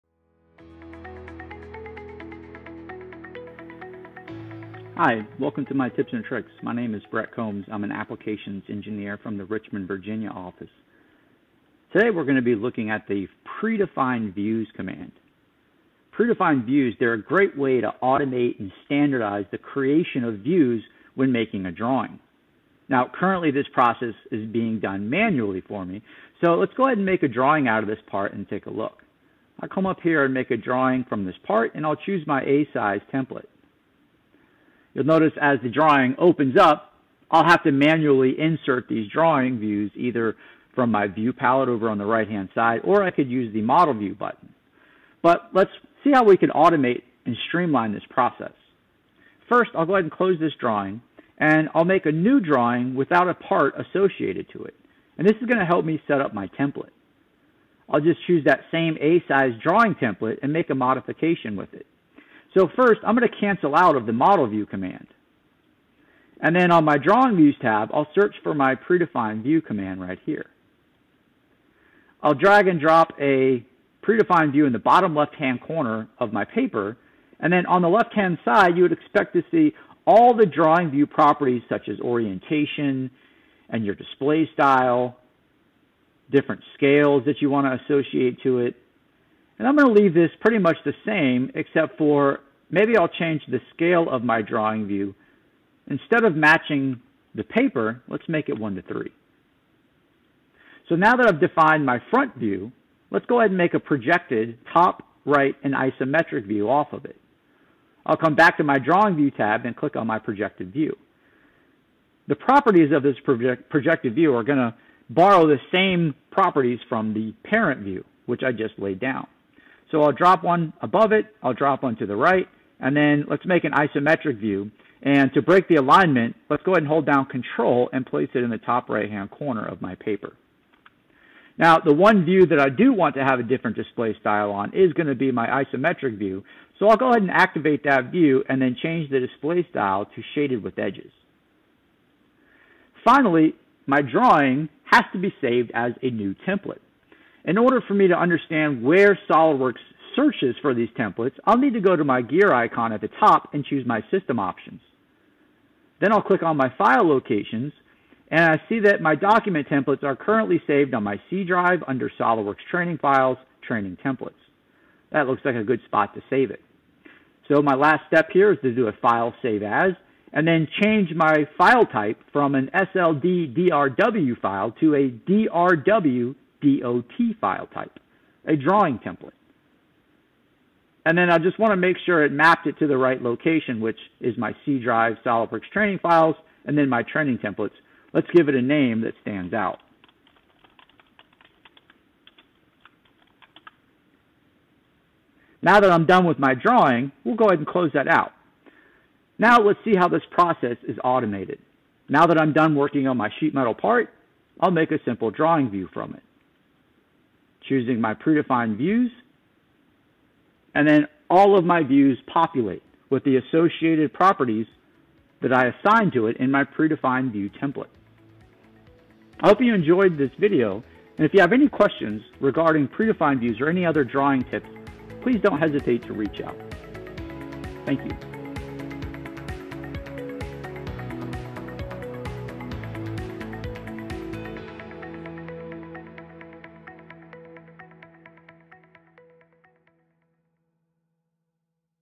Close-up Look at the Predefined View Command in SOLIDWORKS
The predefined view command is a great way automate and standardize the creation of views when making a drawing.